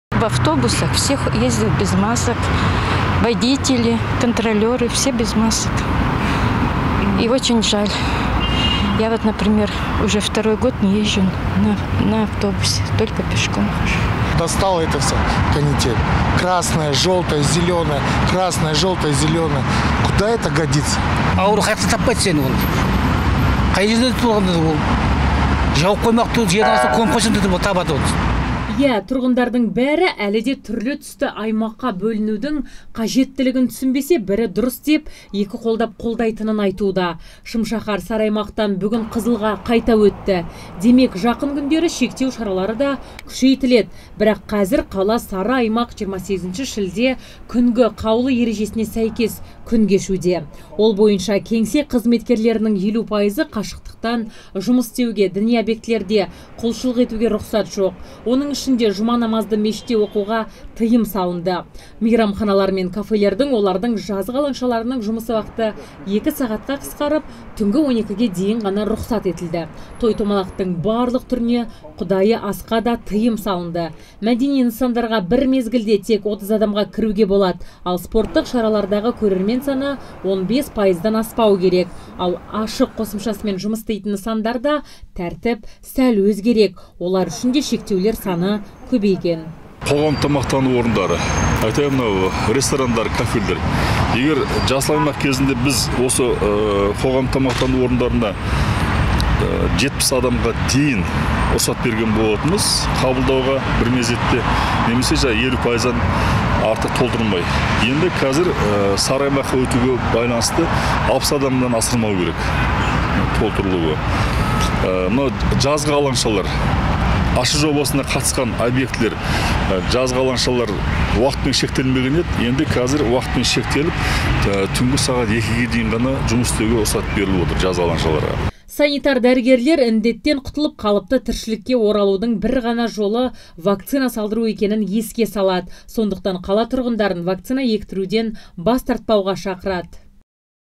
ЭПИДАХУАЛ: «ҚЫЗЫЛ АЙМАҚҚА ҚАЙТА КІРДІК"
1 шілдедегі жағдай бойынша Қазақстан өңірлеріндегі эпидемиологиялық ахуалды бағалау матрицасы жарияланды. «Қызыл» аймаққа өткен аймақтардың қатарында Шымкентте бар. Енді таяуда қаланың бас санитар дәрігерінің қаулысы өзгеріп, көп нысандарға кіріп-шығу шектеледі. Тұрғындар түстерге бөлінудің тіршілікке әсері бар-жоғы туралы не дейді?